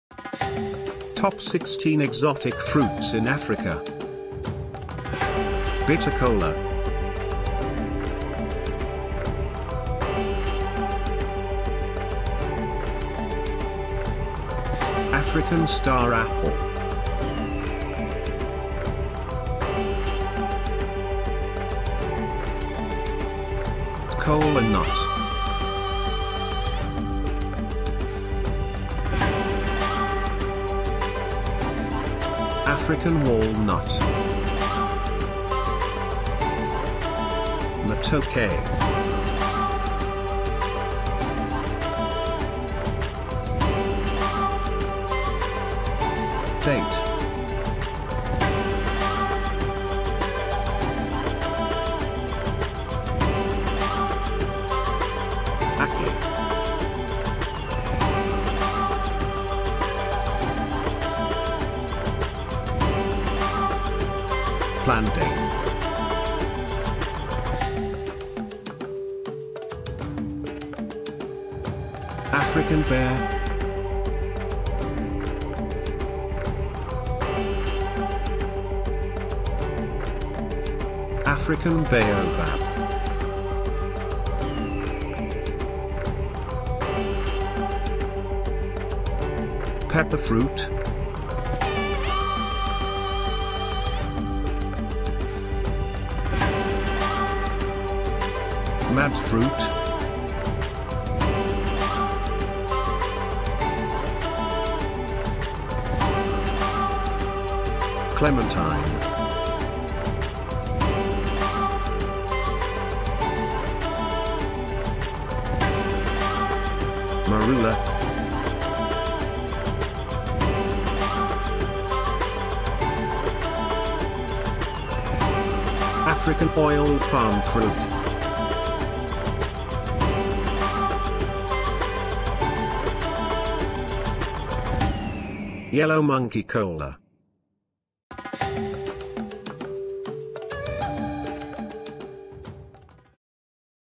TOP 16 EXOTIC FRUITS IN AFRICA
Bitter Kola
African Star Apple
Kola Nut
African Walnut
Matoke
Date
Ackee
Plantain
African Pear
African Baobab
Pepper Fruit
Madd Fruit
Clementine
Marula
African Oil Palm Fruit
Yellow Monkey Kola